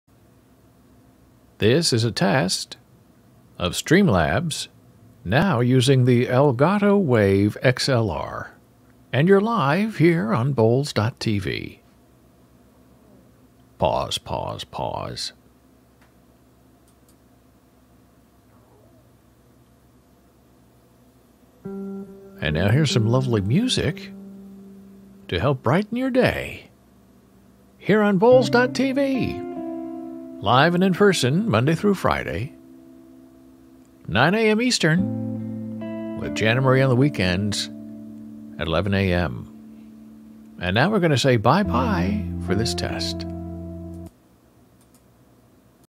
Streamlabs Audio Test: Elgato Wave XLR
Audio streaming test -- with video! -- for the Elgato Wave XLR.